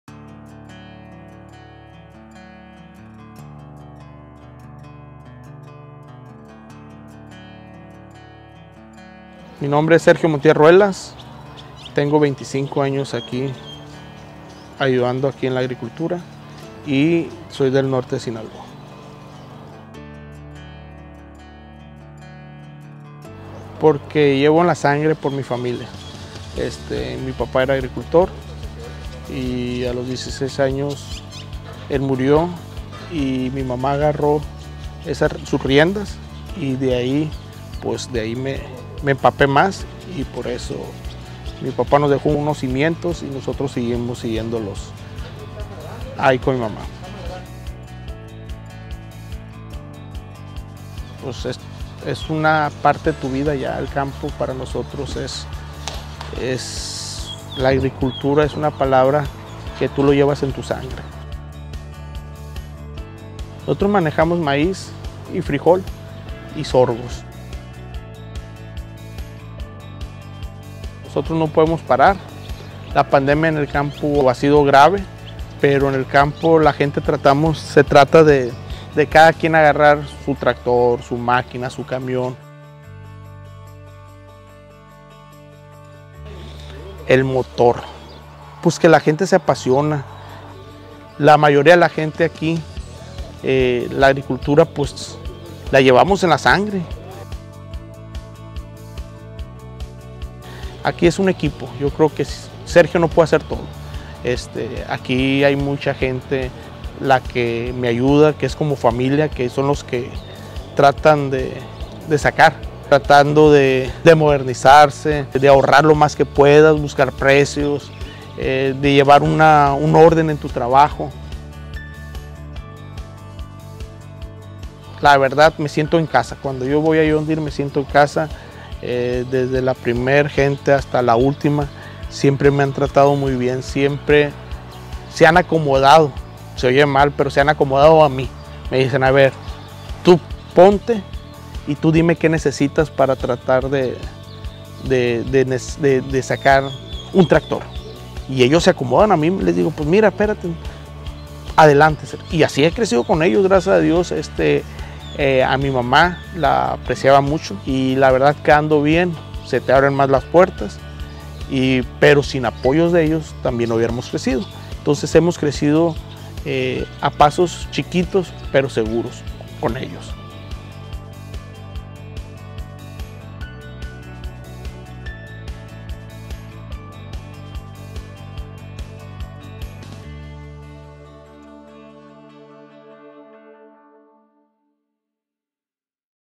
Testimonial Sergio Montiel
Detrás de cada tractor, hay un agricultor con una historia de esfuerzo y dedicación por la tierra. En esta edición de Expo Agroalimentaria, John Deere rinde homenaje a quienes trabajan la tierra con grandes promociones.